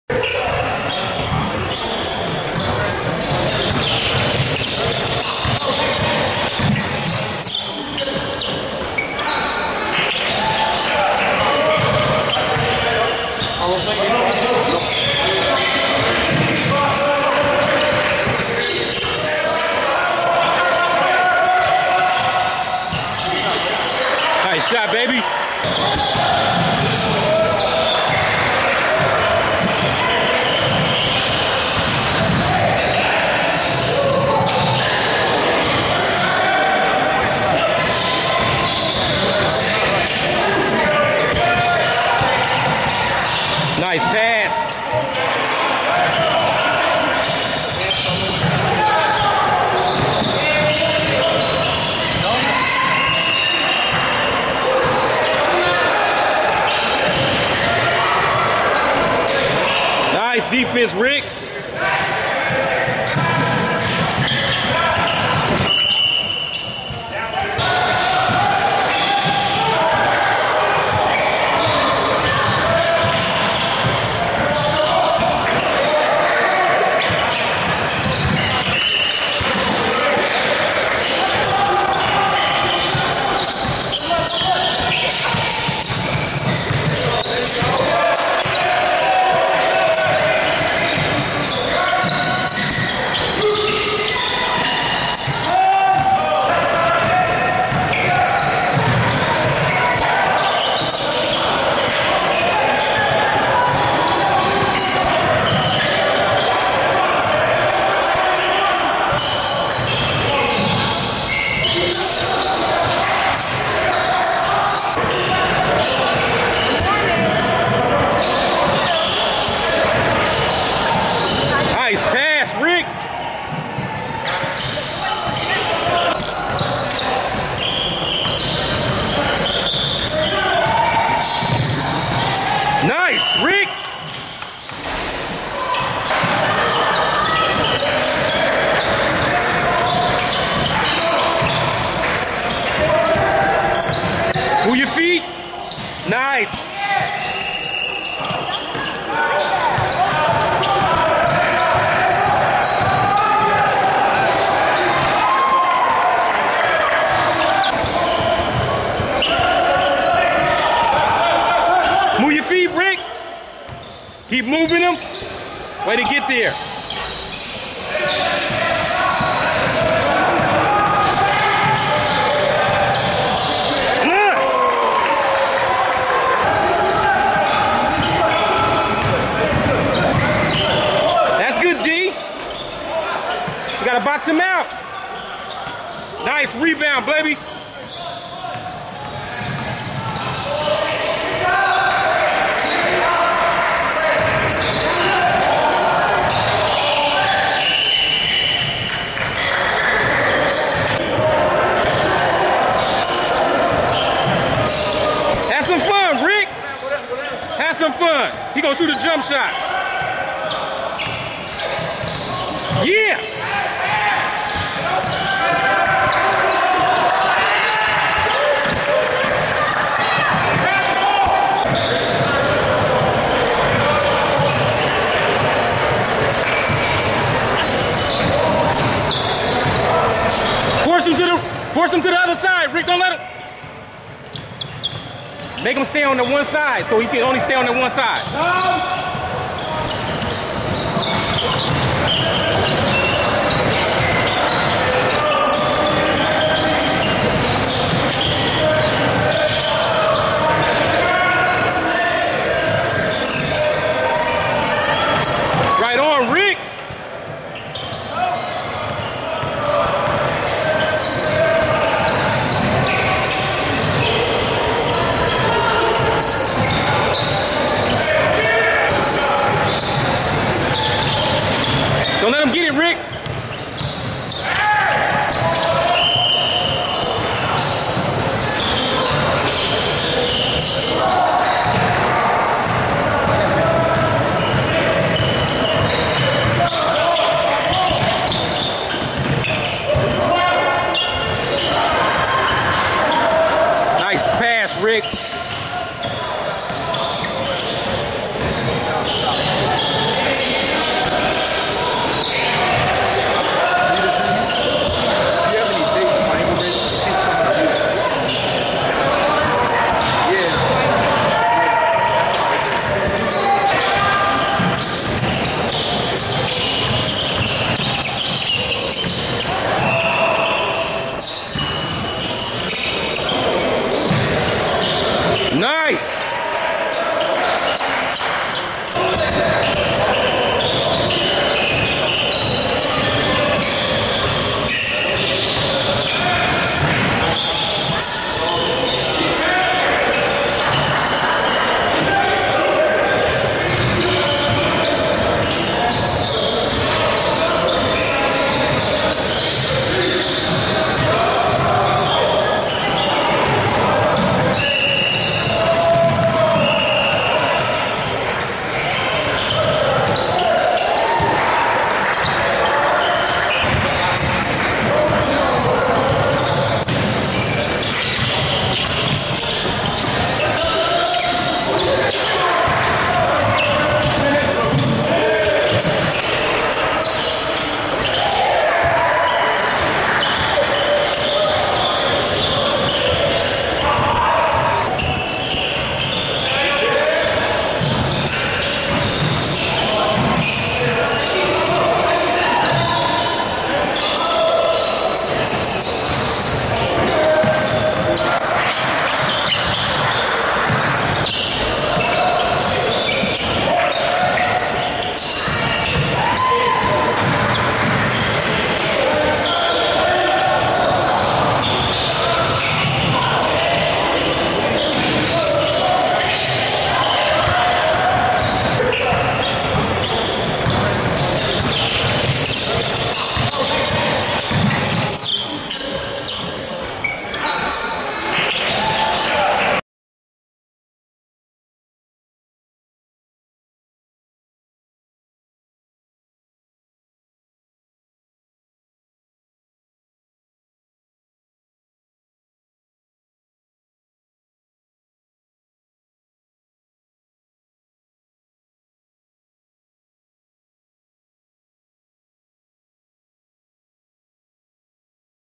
Ricky Bell 3rd RB3 2020 PG Game HL 10 pt, 6 ast, 6 reb, 1 stl Charlottesville Virgina SF Showcase Ga
Ricky Bell 3rd RB3 2020 (PG) Game HL 10 pt, 6 ast, 6 reb, 1 stl Charlottesville Virgina SF Showcase Game Three. June 16 2017